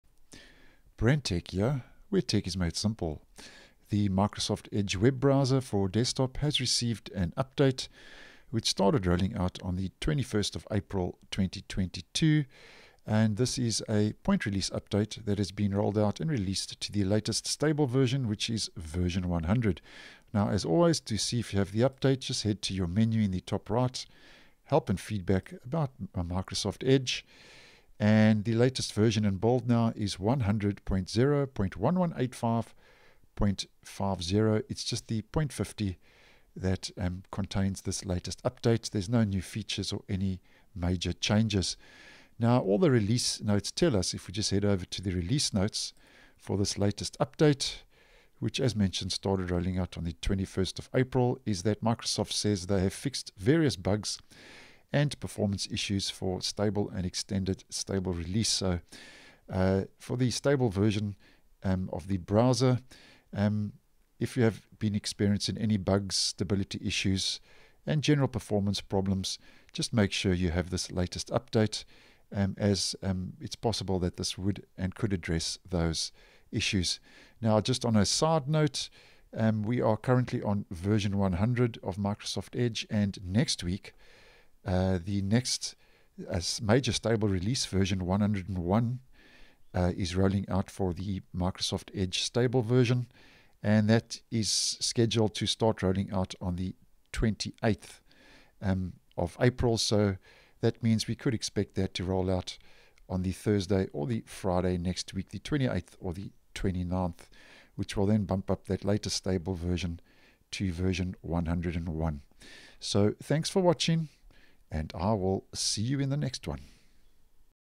Microsoft Edge 100.0.1185.50 update includes bug and performance fixes version 101 coming next week
If you have experienced any issues in the browser, check that it is up to date.